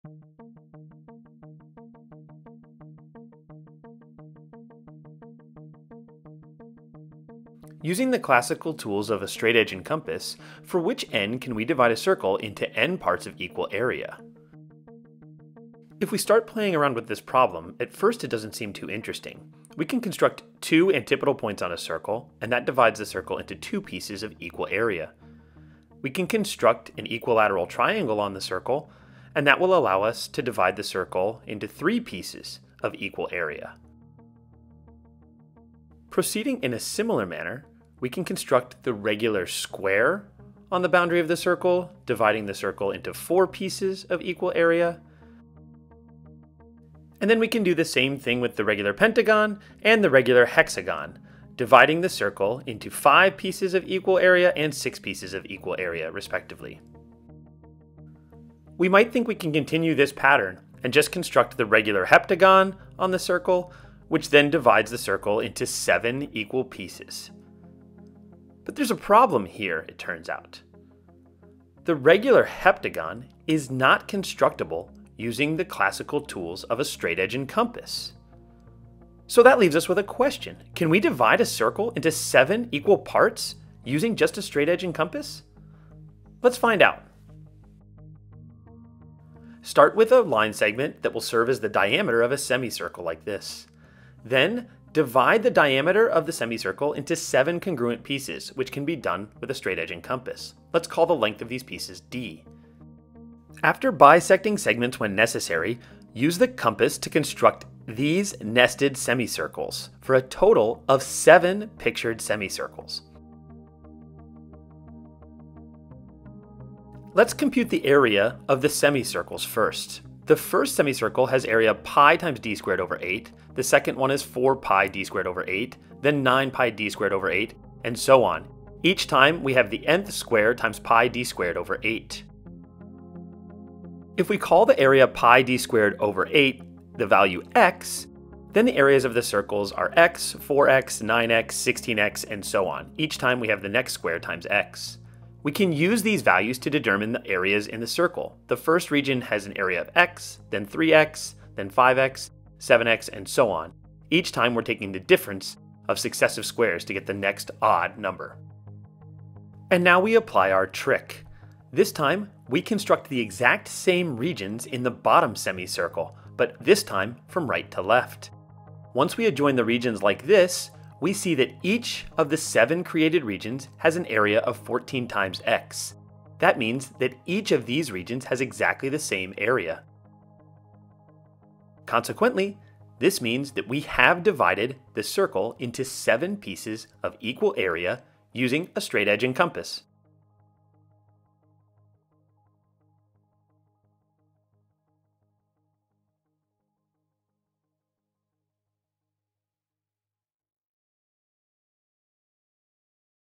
Dividing a Circle into Seven Equal Areas (visual proof)
This is a short, animated proof showing how to decompose a circle into seven pieces each with the same area. This process can be done using ruler and compass constructions, unlike the process of constructing the regular heptagon. manim​ mathshorts​ mathvideo​ math​ geometry mtbos​ animation​ theorem​​​ visualproof​ proof​

Perhaps this should be titled "The Best Way to Cut Pizza into 7 pieces" based on the comments in this video :)

There were also a number of comments asking about how to divide a line into seven equal lengths, so I have created a short, wordless animation showing that procedure if you're interested

This animation is based on a visual proof from Roger Nelsen's and Claudi Alsina's wonderful book "Icons of Mathematics" (Chapter 14.1 on page 76).

(I may receive commissions from this paid link) : Icons of Mathematics from Amazon